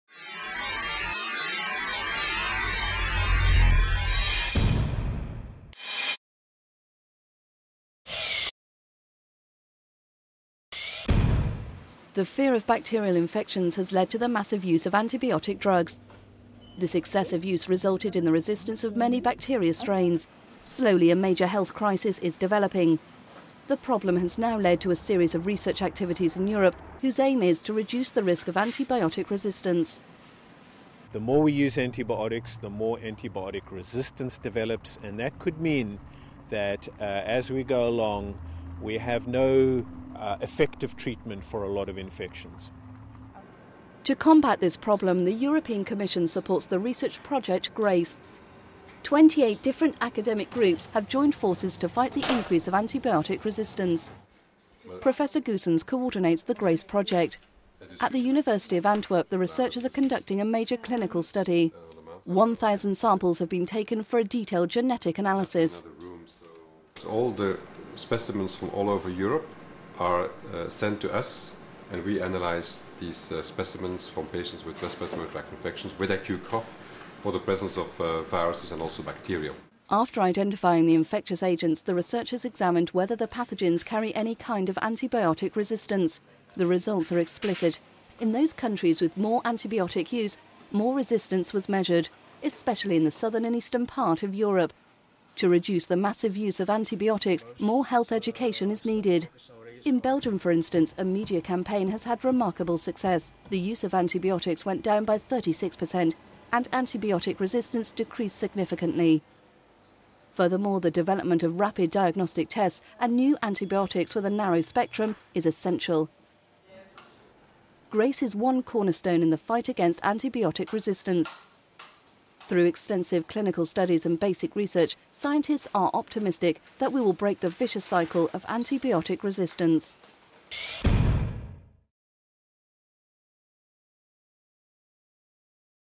Restoring the Magic of a Miracle Drug
More people die of hospital germs than of HIV every year. The reason is that antibiotics are becoming useless against an ever bigger number of multi-resistant bacteria that are spreading throughout the world. Today, this is not just an issue in hospitals, but throughout society at large. After the Second World War penicillin became the first antibiotic drug to be used against bacterial infections. Through intensive use however, penicillin and a lot of second generation antibiotic drugs lost their power. Bacteria developed a resistance against them and now some infections are at risk of becoming as deadly as they were before.